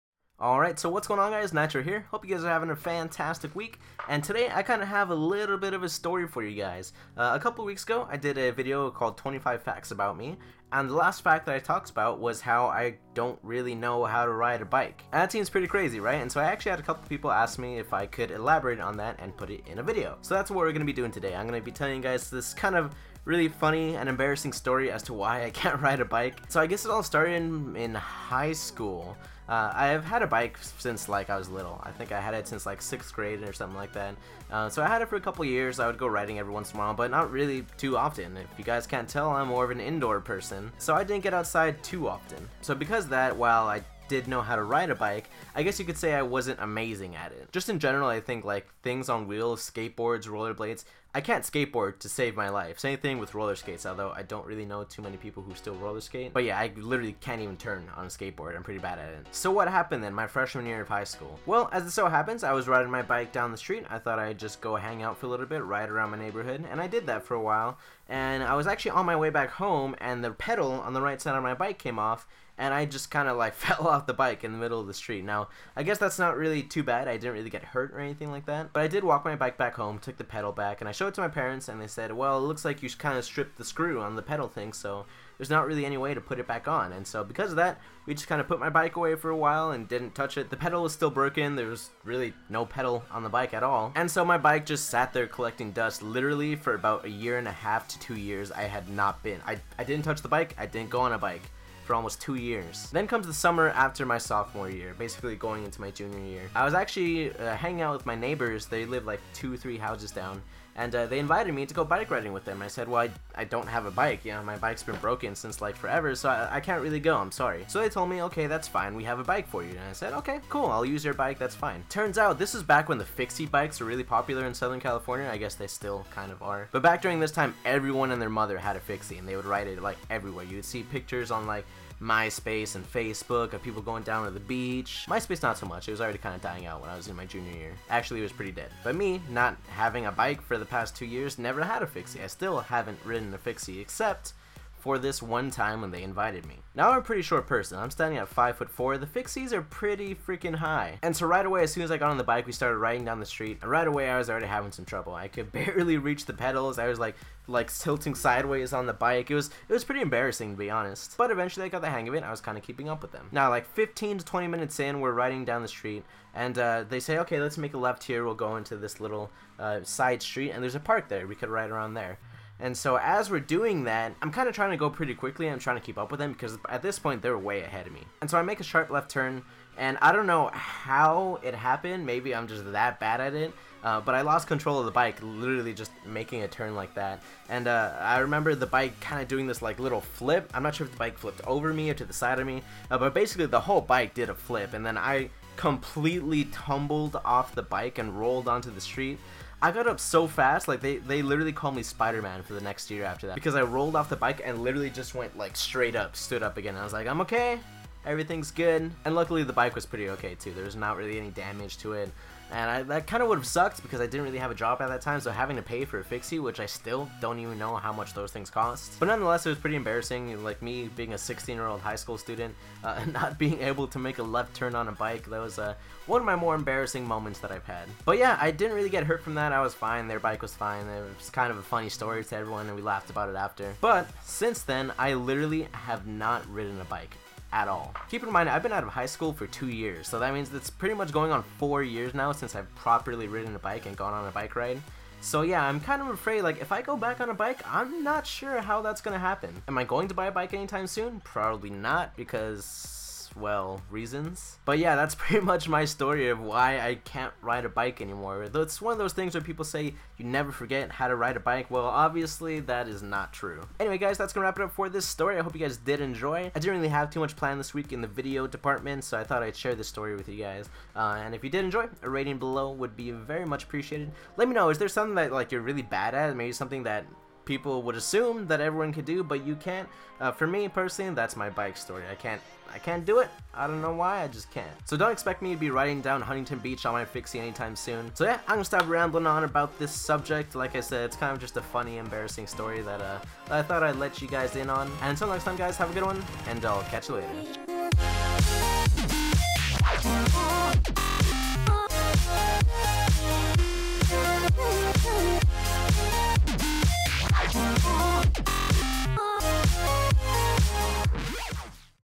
My Bike Story!
Today I share a pretty funny and embarrassing story with you guys. Enjoy!

Snapchat me: misternitro12 (I don't bite! Snap meh.)